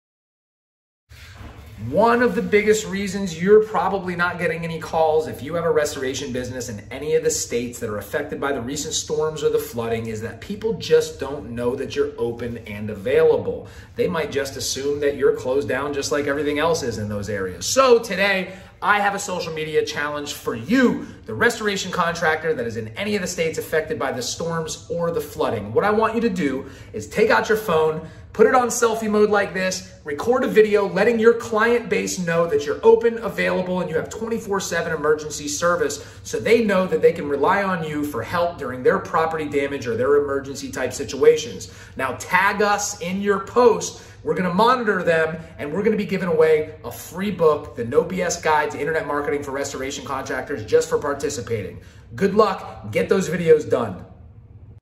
Social media posting challenge. Do you have the guts?
Want to win a free copy of our book "The No Bulls**t Guide To Internet Marketing For Restoration Contractors"? All you have to do is record a video that you are available for emergency service, post it on your social media account, tag us, and we'll send you a copy of our book.

Want to take your restoration business to the next level? Just click on the link below to schedule a FREE strategy session today.